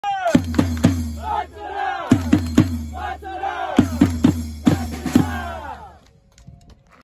2023テ宮J3 28節、松田浩(元)監督コール。
2023/9/23、テゲバジャーロ宮崎はアウェー岩手県にて
いわてグルージャ盛岡戦でした。いわぎんスタジアムにて。